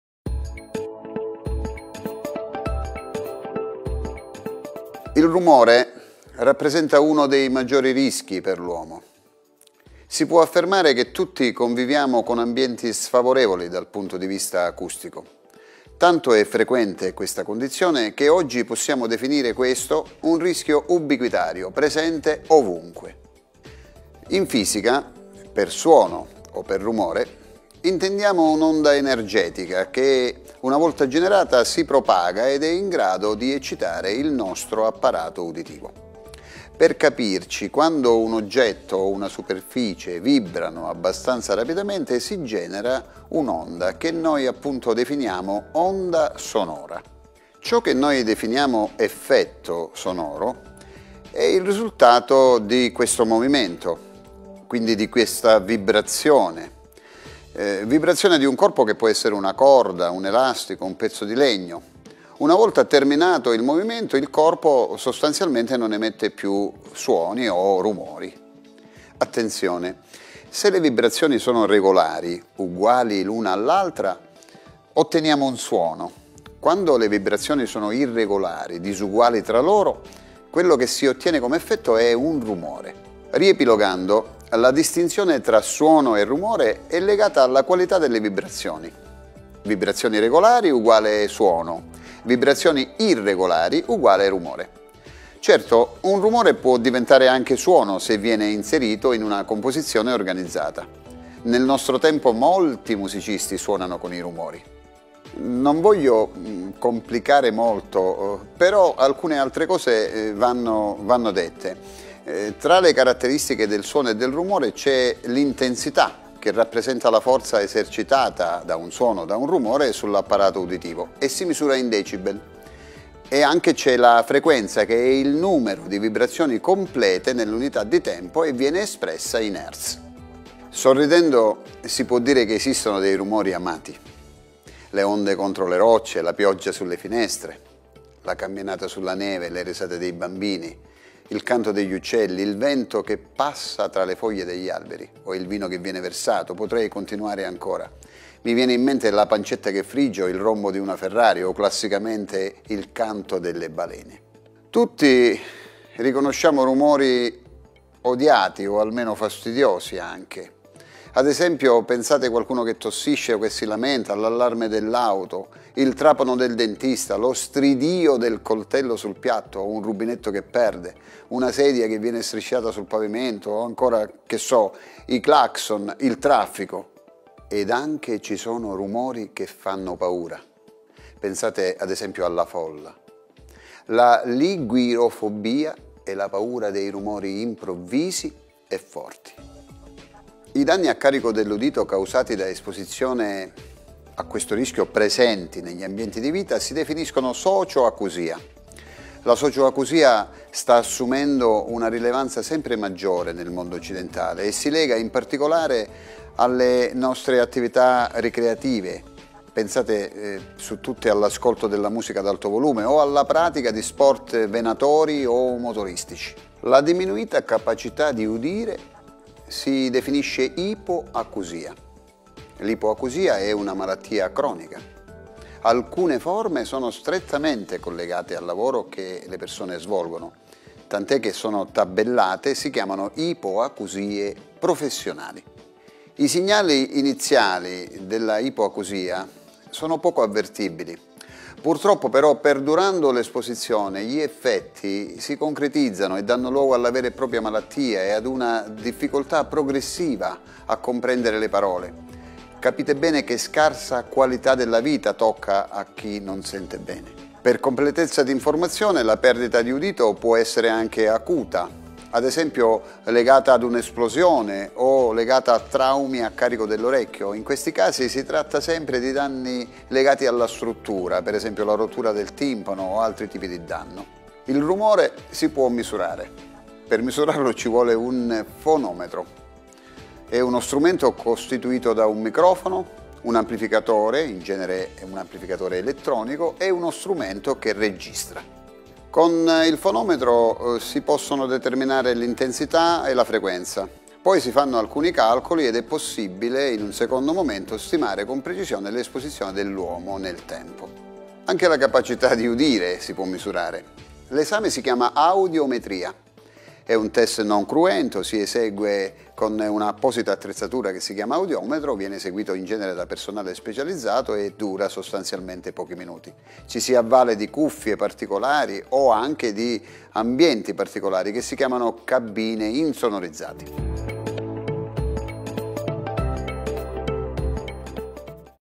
Salute e ambiente. Il rumore
Cos'è il rumore, come si misura, le patologie che causa.
Il Dott. Mario Lazzaro, Medico del Lavoro, ne illustra le caratteristiche e le conseguenze.